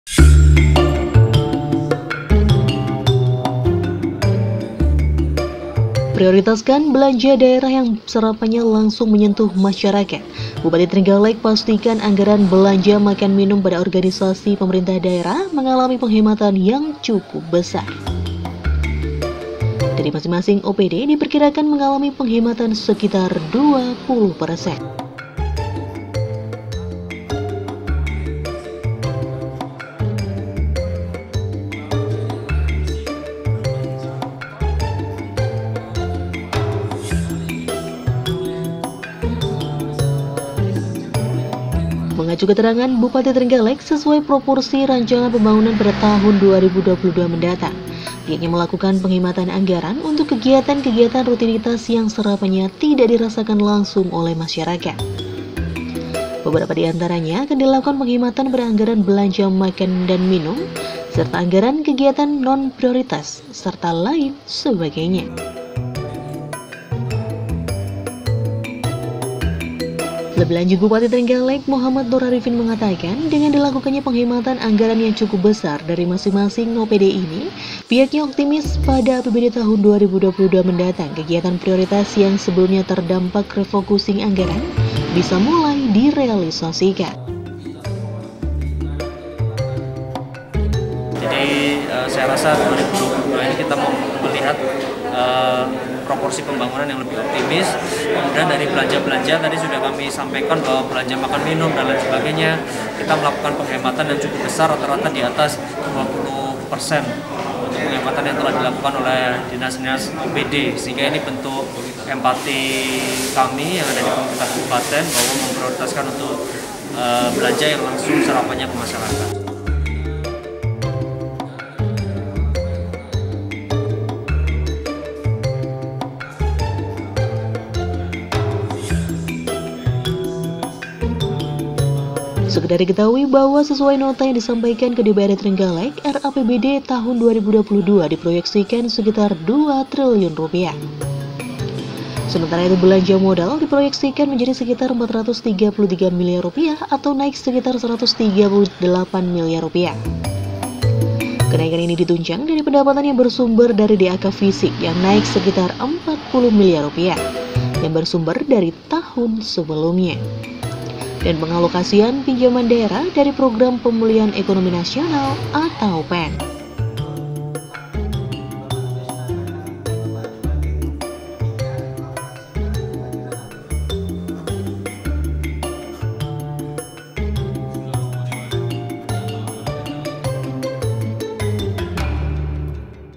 R APBD 2022, Bupati Trenggalek Pastikan Belanja Makan Minum OPD Berkurang Sekitar 20% - bioztv.id
R APBD 2022, Bupati Trenggalek Pastikan Belanja Makan Minum OPD Berkurang Sekitar 20%

- Prioritaskan belanja daerah yang serapannya langsung menyentuh masyarakat, Bupati Trenggalek pastikan anggaran belanja makan minum pada organisasi pemerintah daerah (OPD) mengalami penghematan yang cukup besar. Dari masing masing OPD diperkirakan mengalami penghematan sekitar 20%.

Mengacu keterangan Bupati Trenggalek, sesuai proporsi rancangan pembangunan pada tahun 2022 mendatang, pihaknya lakukan penghematan anggaran untuk kegiatan kegatan rutinitas yang serapannya tidak dirasakan langsung oleh masyarakat. Beberapa diantaranya akan dilakukan penghematan pada anggaran belanja makan minum, anggaran kegiatan non prioritas, dan lain sebagainya.

Lebih lanjut Bupati Trenggalek, Mochamad Nur Arifin menyampaikan, dengan dilakukannya penghematan anggaran yang cukup besar dari masing masing OPD ini, pihaknya optimis pada APBD Tahun 2022 mendatang kegiatan prioritas yang sebeleumnya terdampak refokusing anggaran bisa mulai direalisasikan.

Sekedar diketahui bahwa, sesuai nota yang disampaikan ke DPRD Trenggalek, R-APBD 2022 diproyeksikan sekitar 2 triliun. Sementara itu belanja modal  diproyeksikan menjadi sekitar 433 Miliar Rupiah atau naik sekitar  138 Miliar rupiah. Kenaikan ini ditunjang dari pendapatan yang bersumber dari DAK fisik yang naik sekitar 40 miliar Rupiah yang bersumber dari tahun sebelumnya, dan pengalokasian pinjaman daerah dari program pemulihan enonimi nasional (PEN).